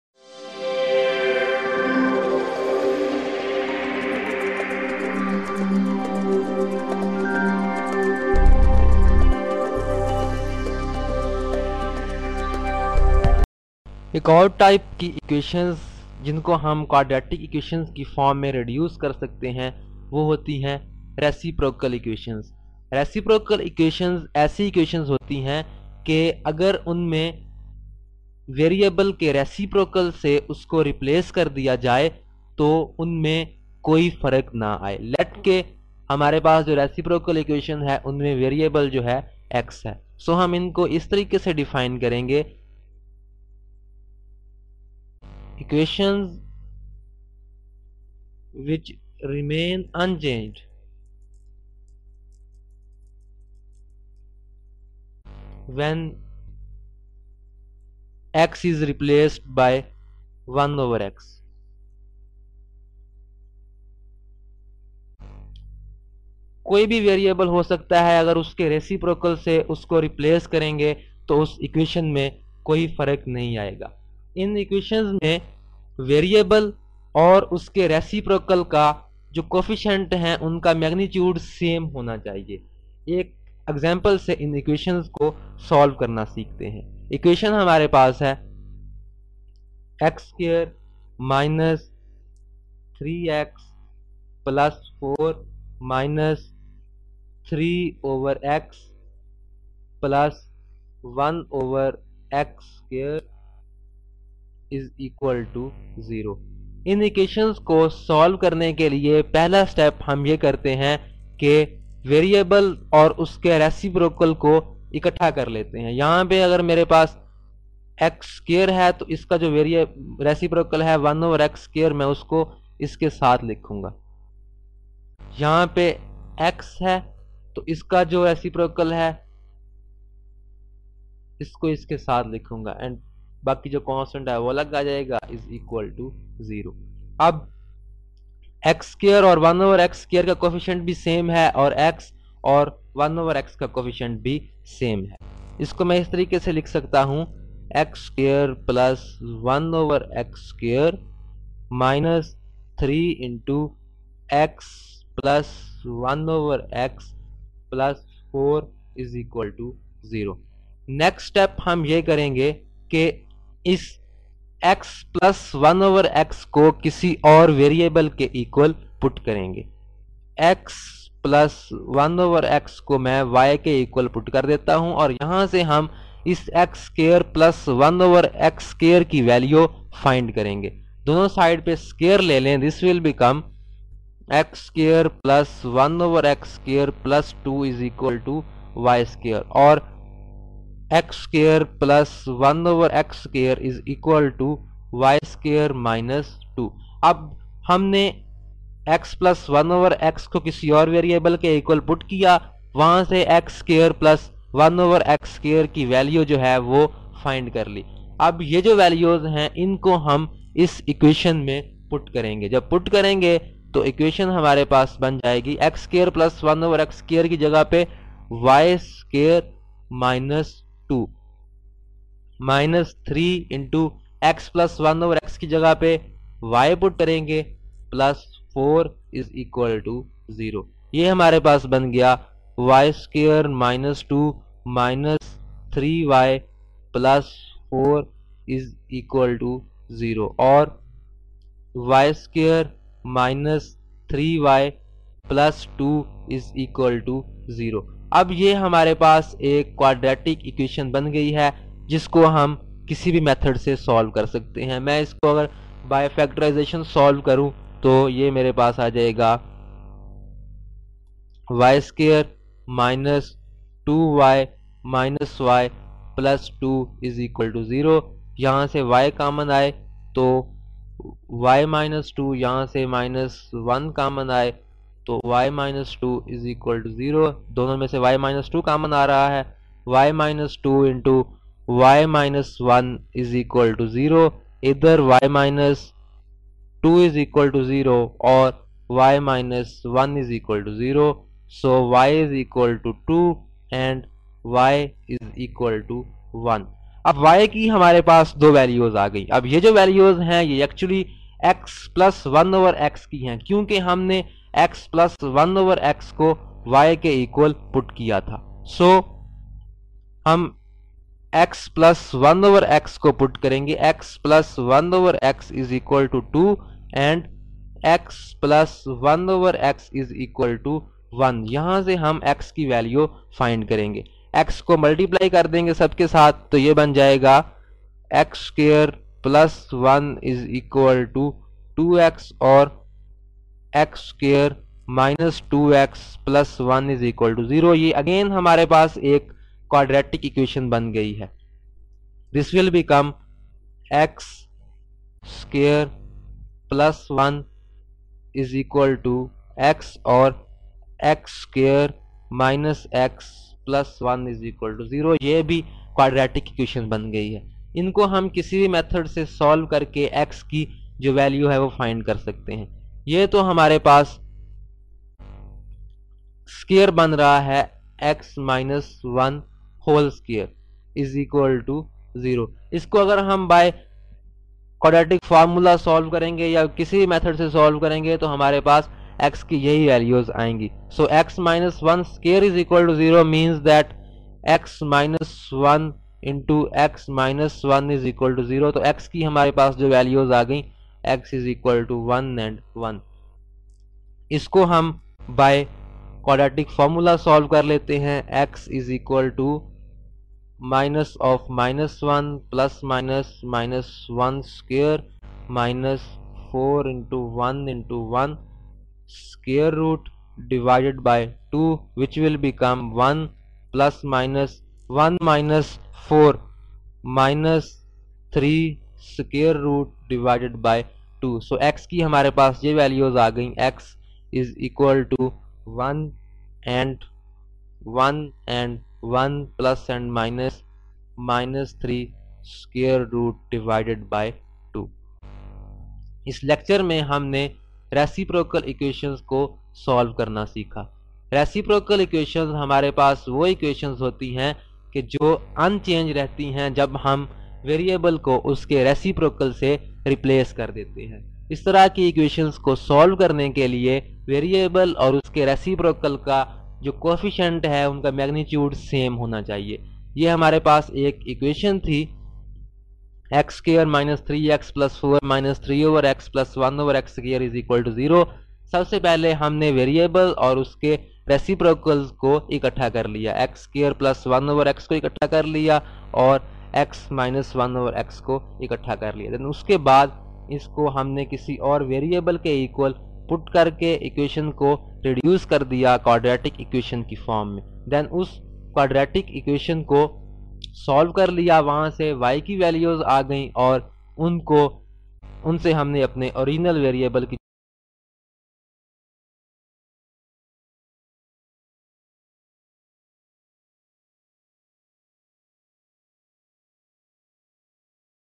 FSc Math Book1, CH 4, LEC 7: Reciprocal Equations
This video lecture from Quadratic Equations (F.Sc. first year Mathematics) covers: The reduction of reciprocal equations to quadratic equations. In such equations we divide the whole equation by some power of variable so that we can get the variable and its reciprocal, Then  we combine the variable and its reciprocal equal to another variable and make a quadratic equation. Find more e-learning material and educational video lectures in Urdu at maktab.pk. These videos are free to use for promotional and commercial purpose by keeping the credits to Maktab.